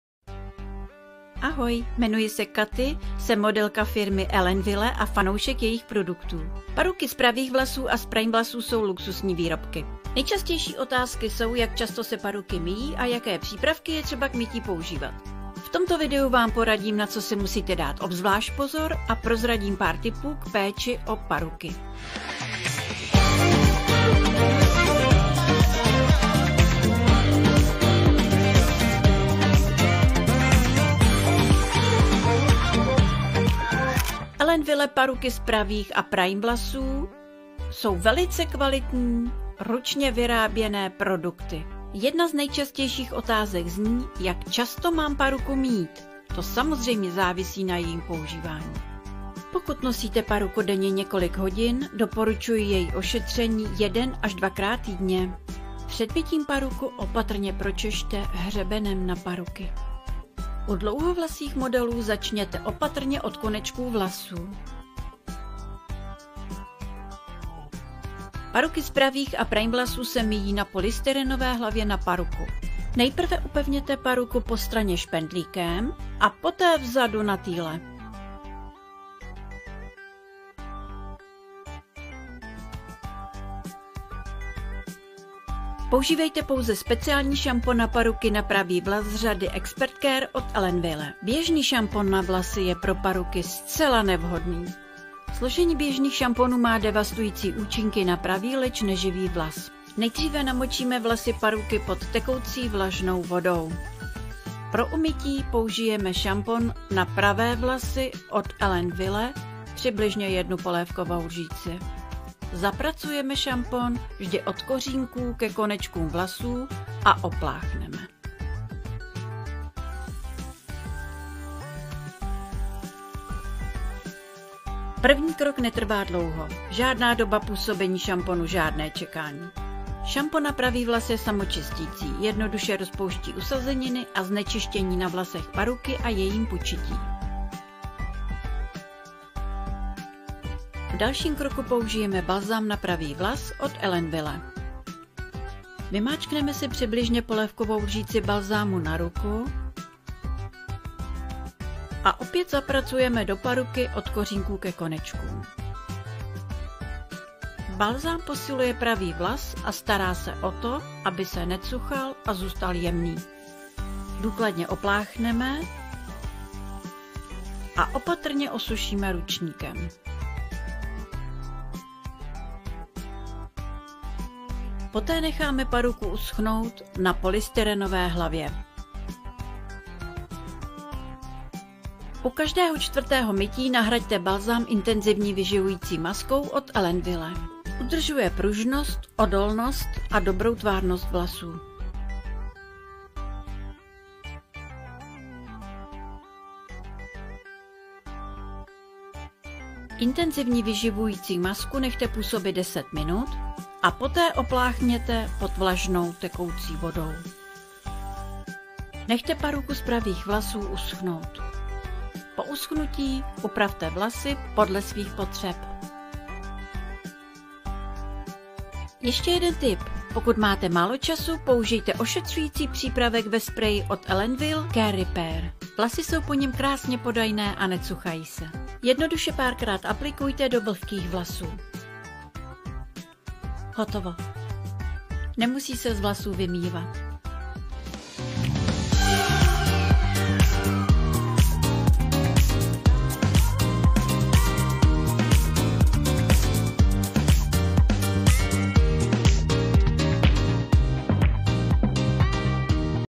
Jak pečovat o paruky
Jak pečovat o paruky z pravých vlasů. A pokud chcete skoro stejnou kvalitu paruk z pravých vlasů tak se podívejte tady: